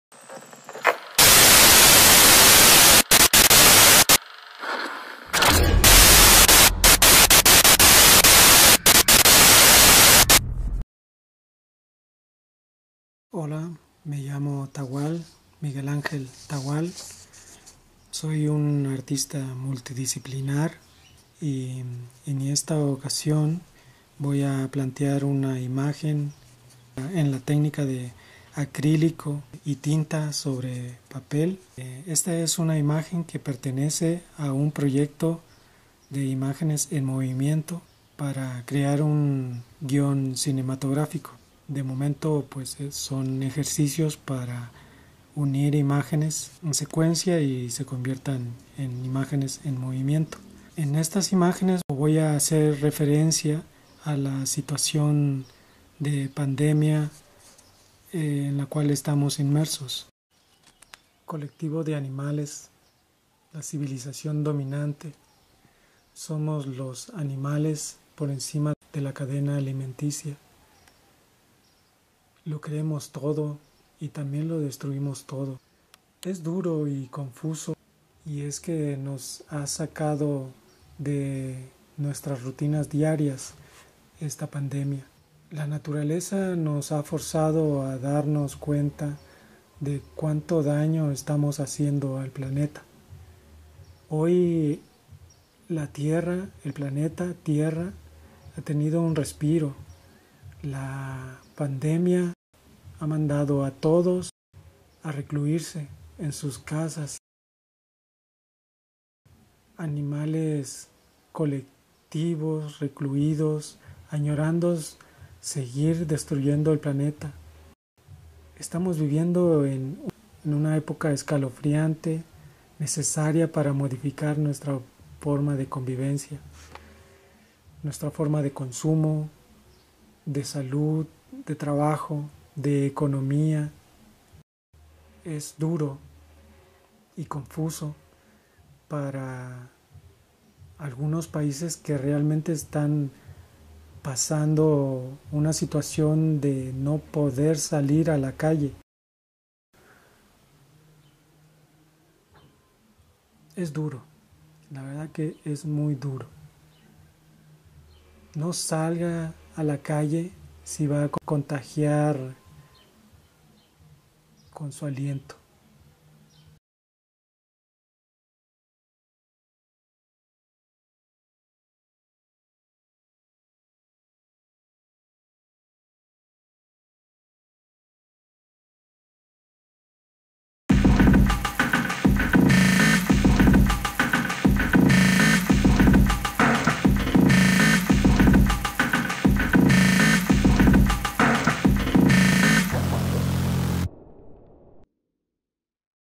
TAHUAL | COLECTIVO DE ANIMALES
Colectivos  de Animales es un pieza desarrollada en tinta y acrilico sobre papel, con unas medidas de 61 x 90 cm.  La imagen describe a la humanidad en decadencia.
Animal Collectives is a piece developed in ink and acrylic on paper, measuring 61 x 90 cm. The image depicts humanity in decline.

covid 19